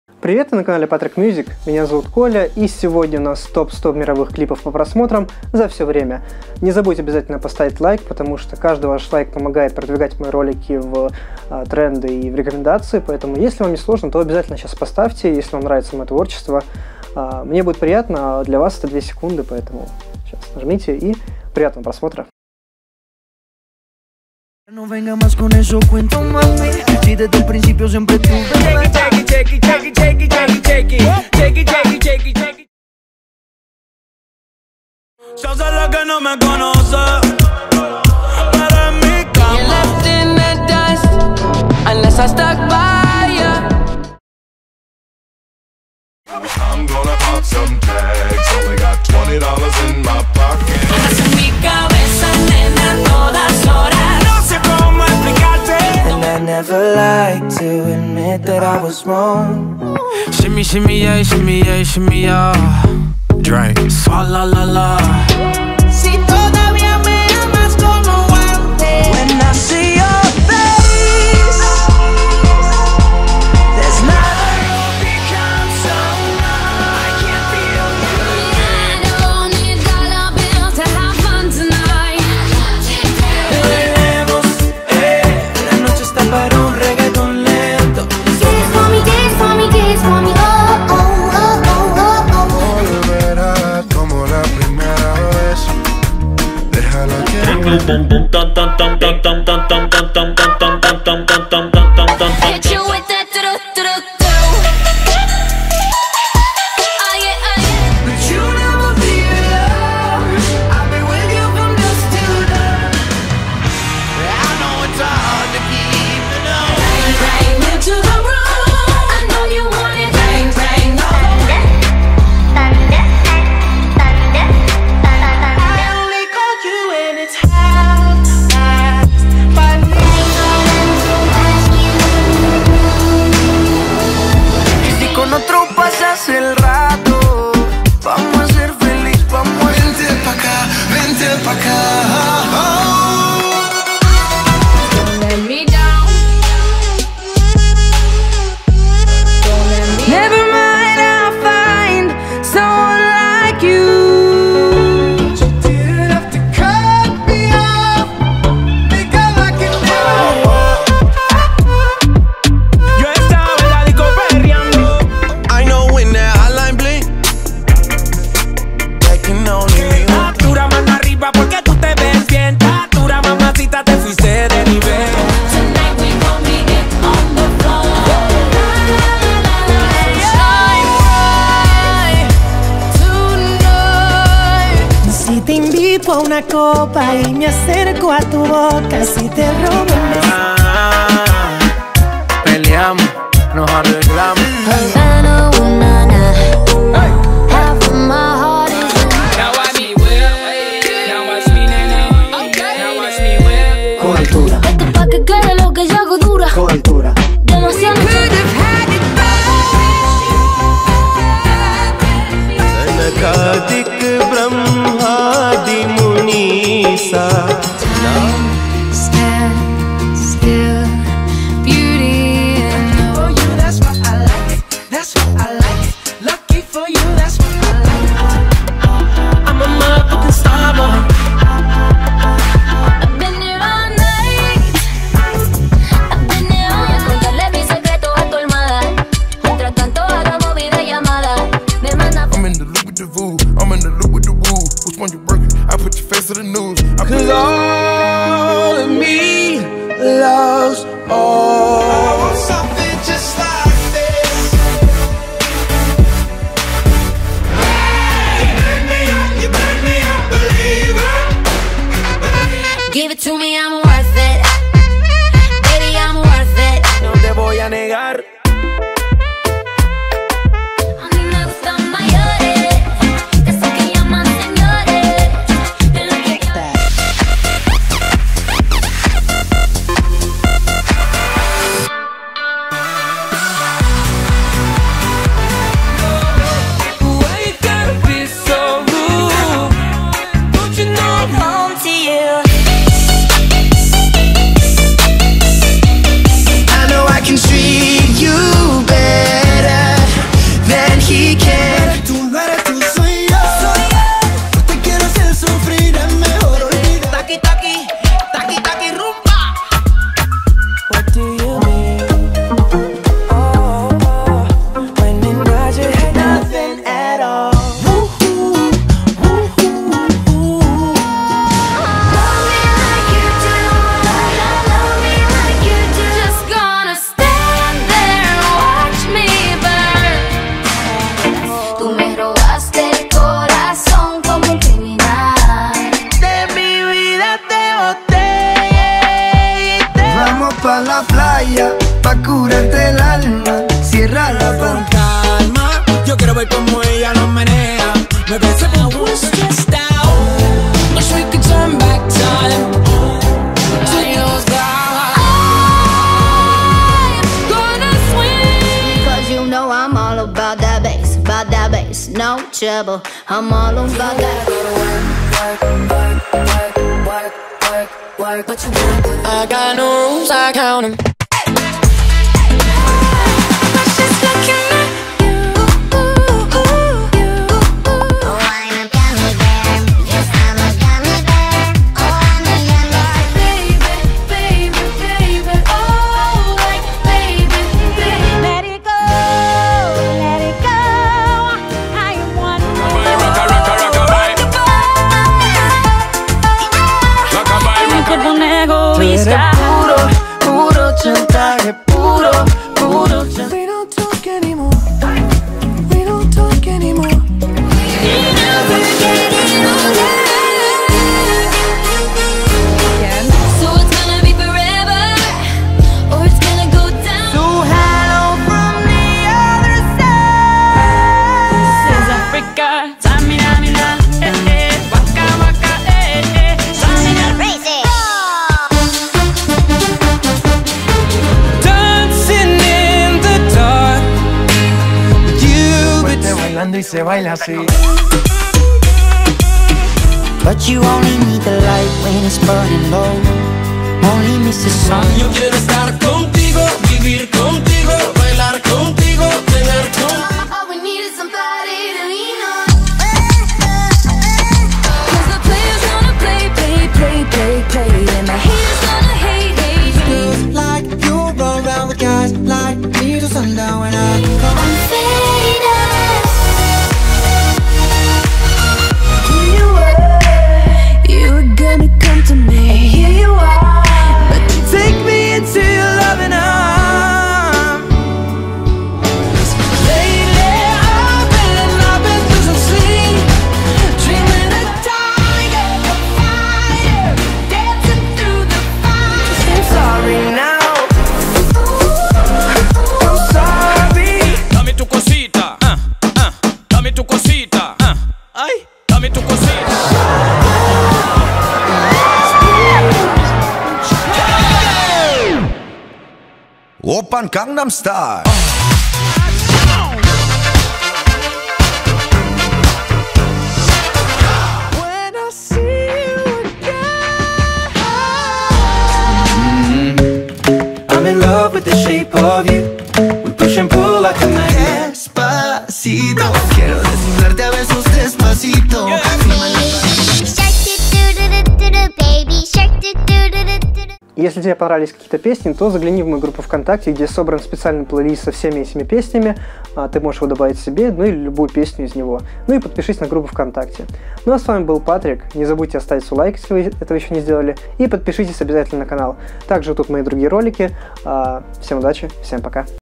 ТОП 100 МИРОВЫХ КЛИПОВ по ПРОСМОТРАМ 2007-2021 | Лучшие зарубежные песни и хиты | Август 2021
Обновленный ТОП 100 самых просматриваемых клипов с 2007 по 2021 года на Ютубе. Не забудь отметиться лайком, чтобы это видео увидело еще большее количество людей